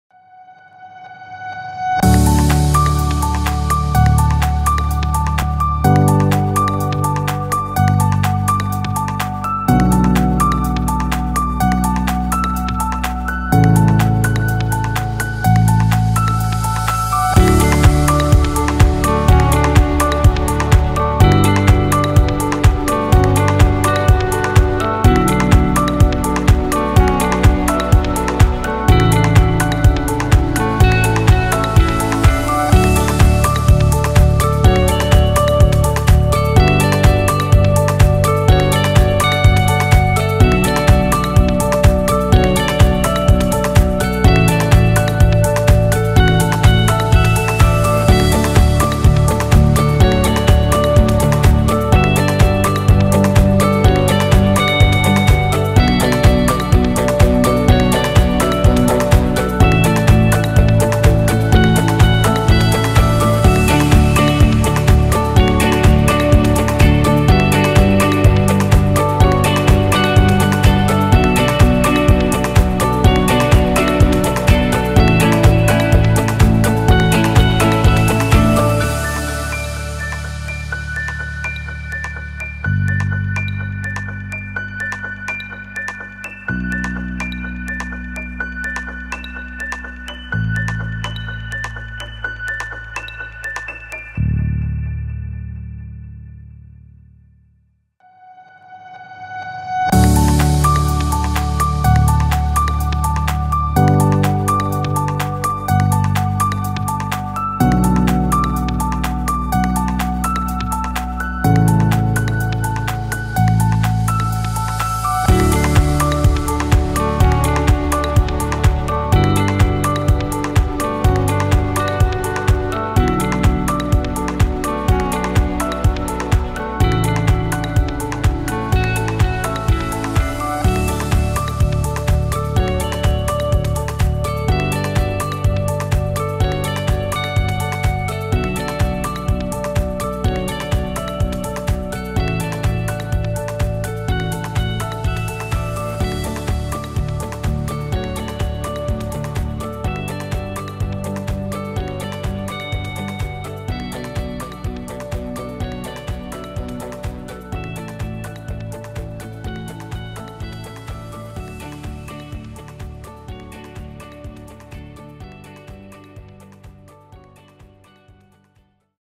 Harbron Home Improvements - Clevedon Hall walk through video
Take a walk with us through our Clevedon Hall resin bound transformation